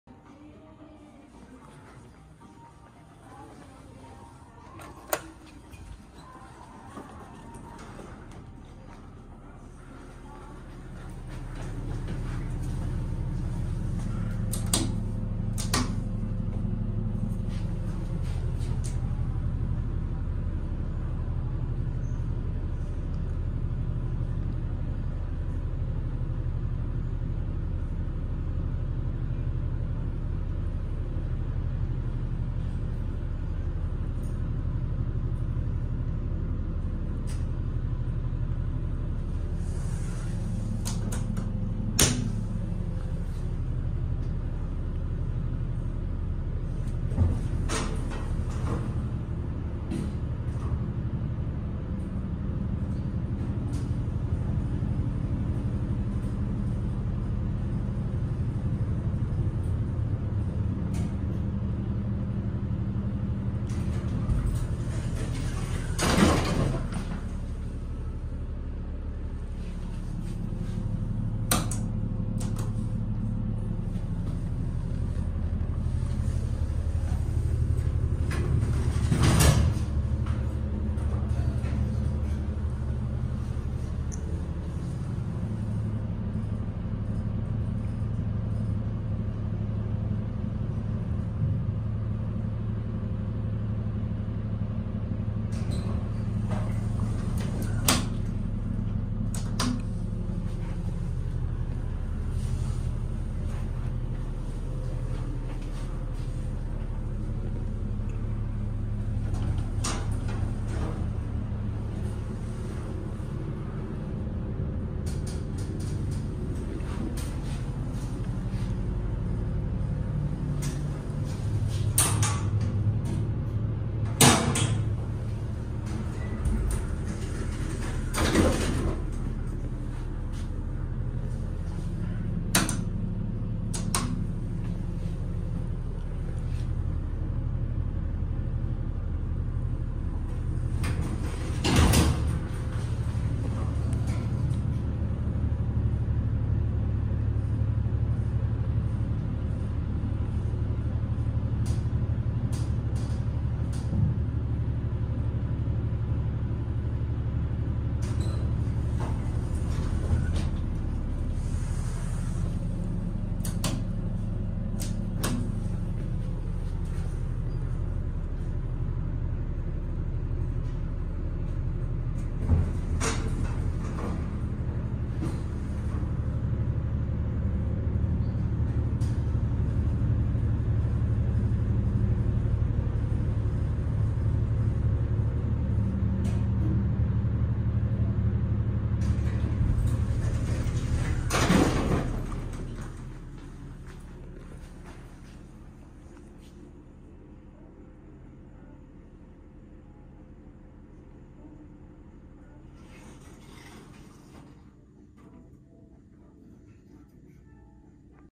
Otis Lexan Elevator At Macy’s At The Oxford Valley Mall In Langhorne Pennsylvania: (05/23/2022)
Monday, 05/23/2022, at 2:58 PM (14:58), this is the elevator at the Macy’s store at the Oxford Valley Mall in Langhorne, Pennsylvania.  This elevator is an Otis Lexan, and it has a maximum capacity inside of 4,000 pounds.